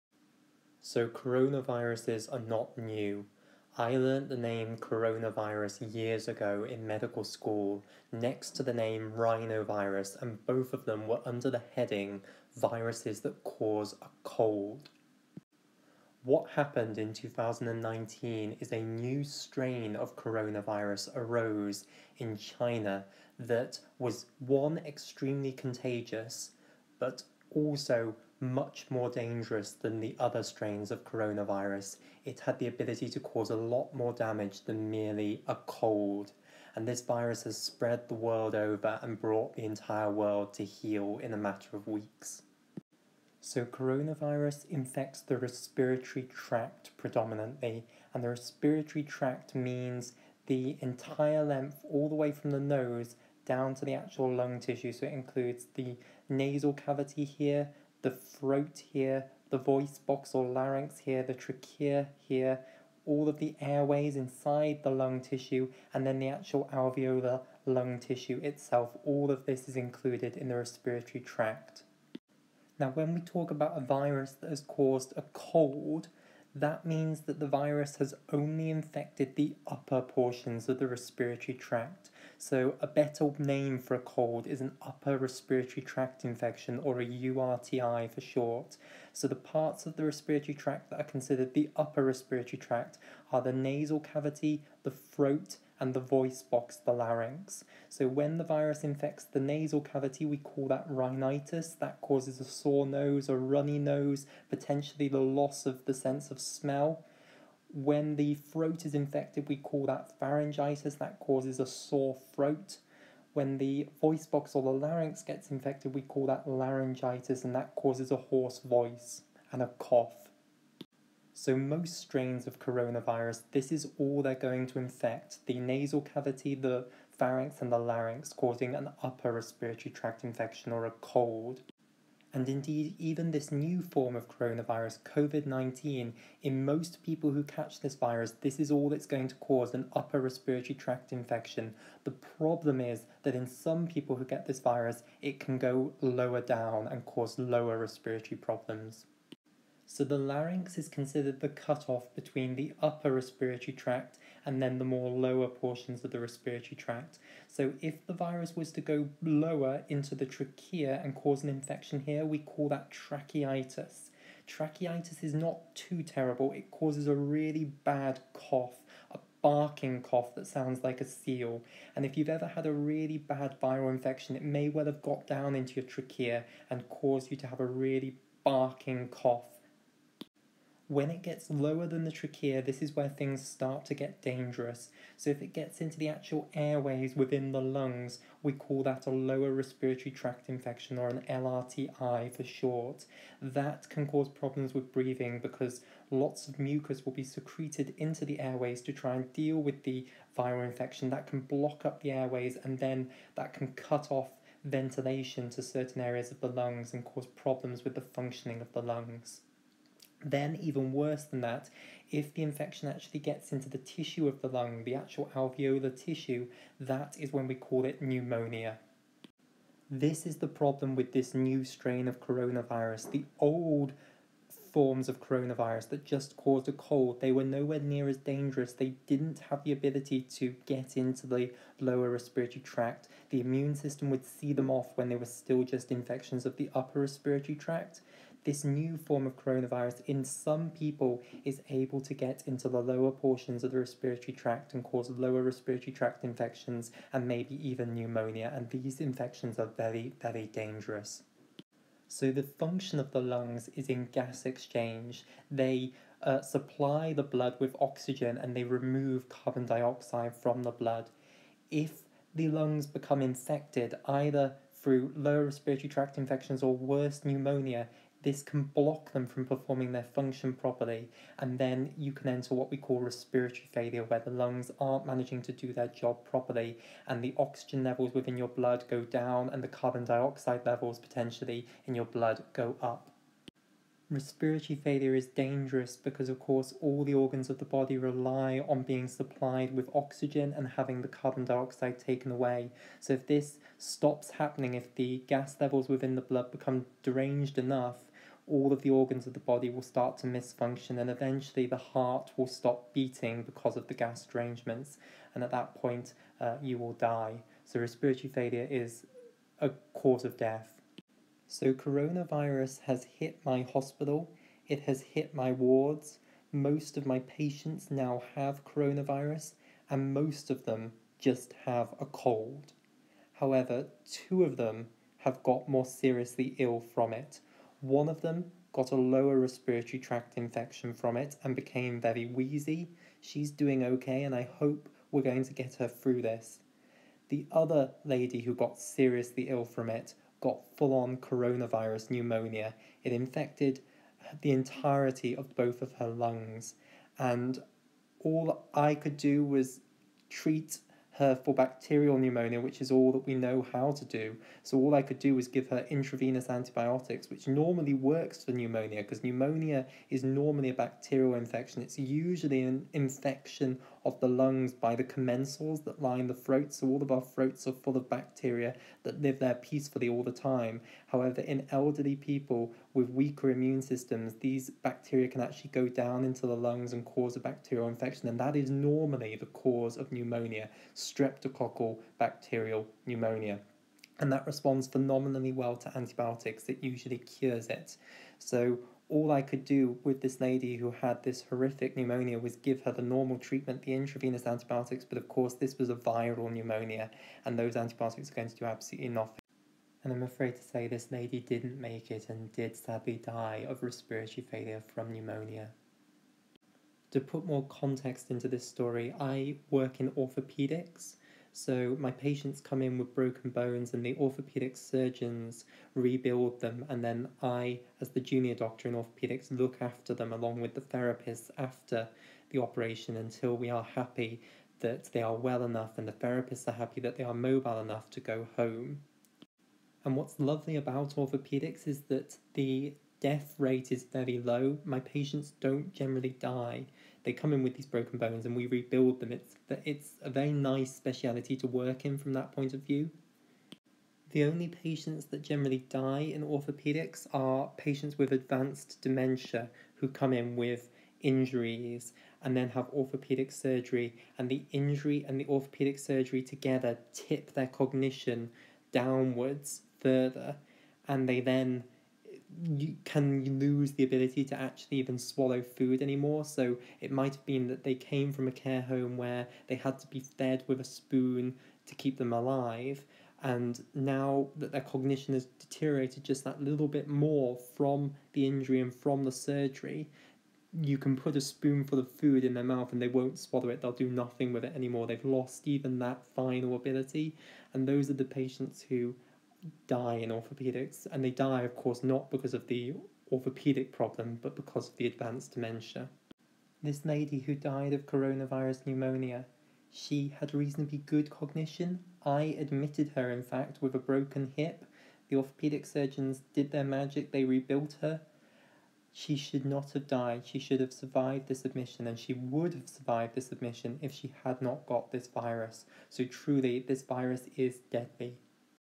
Covid-19
In 2019 a new coronavirus capable of infecting humans emerged. Like other coronaviruses it is predominately a respiratory pathogen. However what makes this virus so deadly is that it has a higher ability to cause pneumonia than the other coronaviruses that commonly circulate and cause only colds. In 2020 it proceeded to spread the world over and brought the whole world to heal.